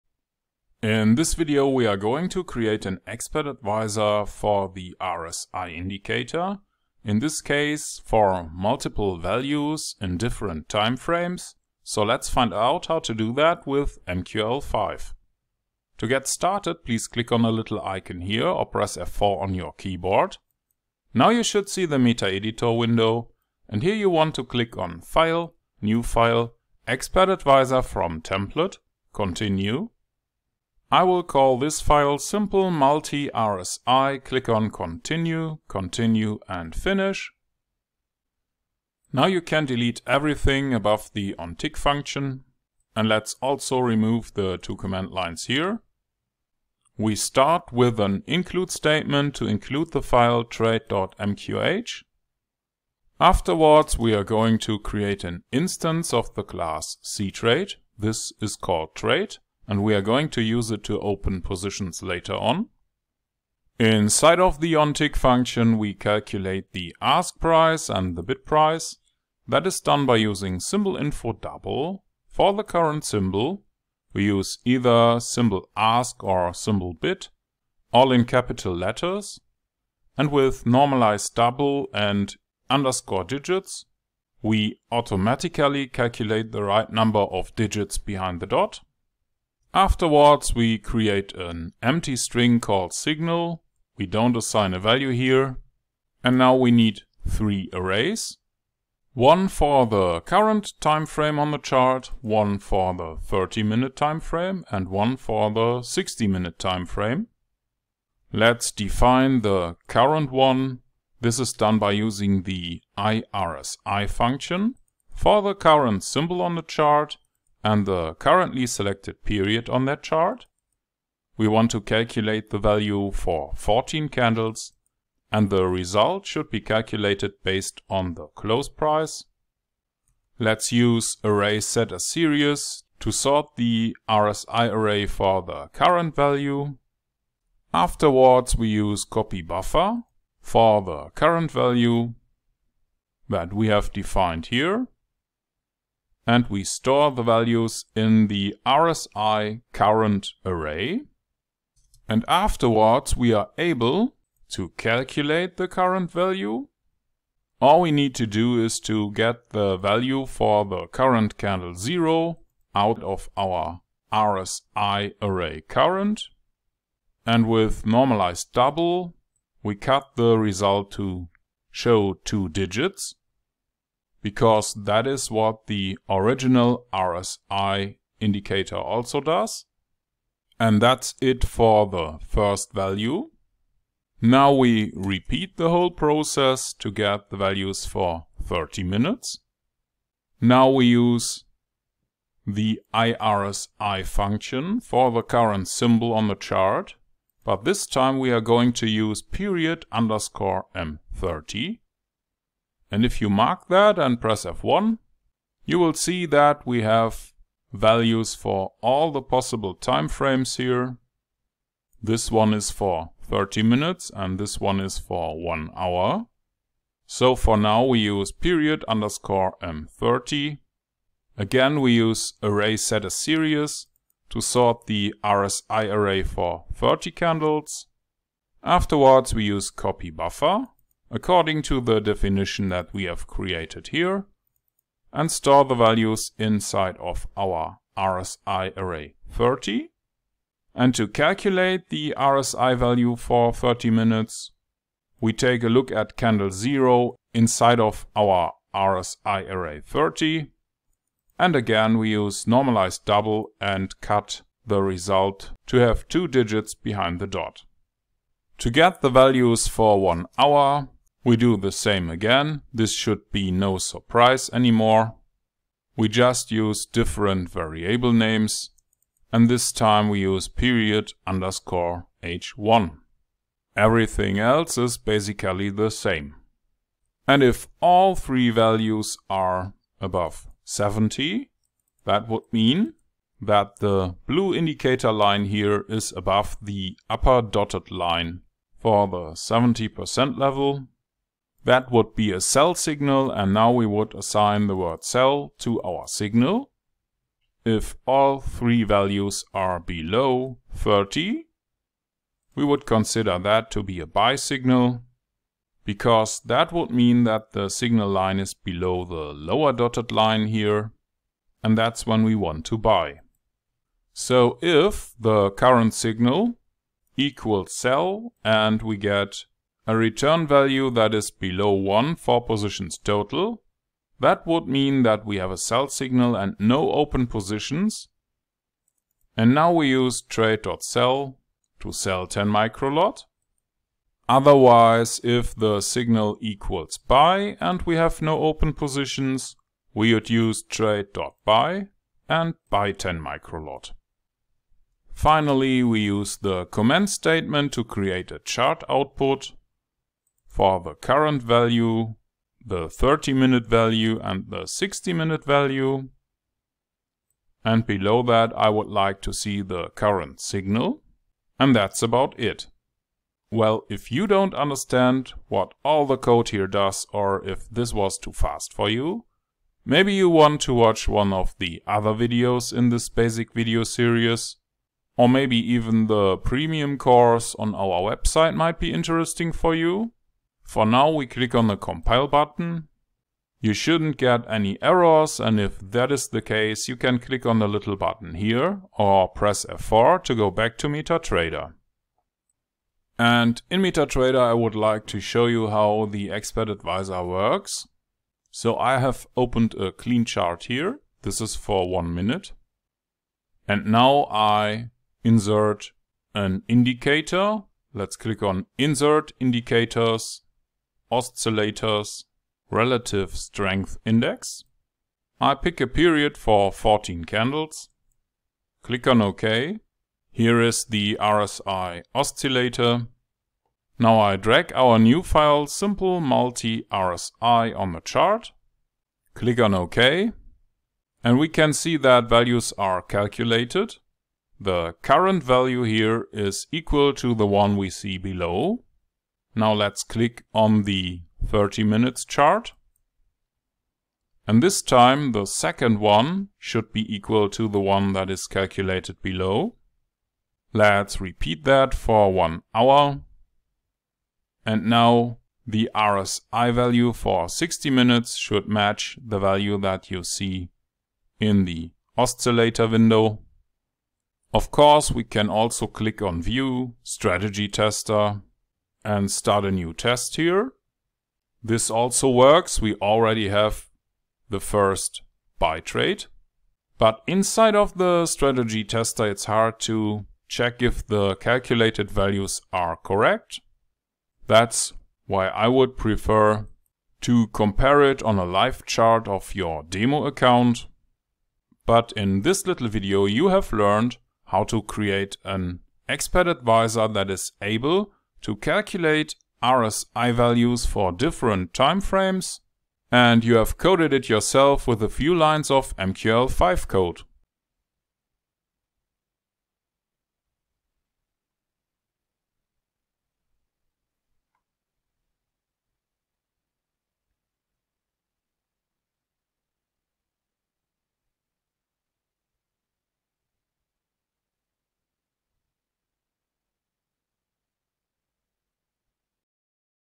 LEARN MQL5 TUTORIAL BASICS - 64 SIMPLE MULTI RSI
With MQL5 for Metatrader5 we create an Expert Advisor to trade the Relative Strenght Index that is also known as RSI Oscillator and calculate the values for different time frames.

In this video we are going to create an expert advisor for the RSI indicator, in this case for multiple values in different time frames, so let's find out how to do that with mql5.
To get started please click on a little icon here or press F4 on your keyboard, now you should see the Metaeditor window and here you want to click on file, new file, expert advisor from template, continue, I will call this file simple multi RSI, click on continue, continue and finish.
Now you can delete everything above the ontick function and let's also remove the two comment lines here.
We start with an include statement to include the file trade dot mqh. Afterwards, we are going to create an instance of the class ctrade, this is called trade and we are going to use it to open positions later on.
Inside of the ontick function we calculate the ask price and the bid price that is done by using symbol info double, for the current symbol we use either symbol ask or a symbol bid – all in capital letters – and with normalize double and underscore digits we automatically calculate the right number of digits behind the dot.
Afterwards, we create an empty string called signal, we don't assign a value here and now we need three arrays, one for the current time frame on the chart, one for the thirty-minute time frame and one for the sixty-minute time frame.
Let’s define the current one, this is done by using the iRSI function for the current symbol on the chart and the currently selected period on that chart. We want to calculate the value for fourteen candles and the result should be calculated based on the close price.
Let’s use array set as series to sort the RSI array for the current value, afterwards we use copy buffer for the current value that we have defined here and we store the values in the RSI current array and afterwards, we are able to calculate the current value. All we need to do is to get the value for the current candle zero out of our RSI array current and with normalize double we cut the result to show two digits because that is what the original RSI indicator also does and that's it for the first value.
Now we repeat the whole process to get values for thirty minutes, now we use the iRSI function for the current symbol on the chart but this time we are going to use period underscore m thirty and if you mark that and press F1 you will see that we have values for all the possible timeframes here, this one is for thirty minutes and this one is for one hour, so, for now, we use period underscore m thirty. Again we use array set as series to sort the RSI array for thirty candles, afterwards we use copy buffer according to the definition that we have created here and store the values inside of our RSI array thirty and to calculate the RSI value for thirty minutes we take a look at candle zero inside of our RSI array thirty and again we use normalize double and cut the result to have two digits behind the dot.
To get the values for one hour we do the same again, this should be no surprise anymore we just use different variable names and this time we use period underscore h one, everything else is basically the same.
And if all three values are above seventy that would mean that the blue indicator line here is above the upper dotted line for the seventy percent level, that would be a sell signal and now we would assign the word sell to our signal.
If all three values are below thirty we would consider that to be a buy signal because that would mean that the signal line is below the lower dotted line here and that's when we want to buy.
So if the current signal equals sell and we get a return value that is below one for positions total that would mean that we have a sell signal and no open positions and now we use trade dot sell to sell ten micro lot.
Otherwise, if the signal equals buy and we have no open positions we would use trade dot buy and buy ten micro lot.
Finally, we use the comment statement to create a chart output for the current value, the thirty-minute value and the sixty-minute value and below that, I would like to see the current signal and that's about it!
Well, if you don't understand what all the code here does or if this was too fast for you maybe you want to watch one of the other videos in this basic video series or maybe even the premium course on our website might be interesting for you, for now, we click on the compile button, you shouldn't get any errors and if that is the case you can click on the little button here or press F4 to go back to Metatrader.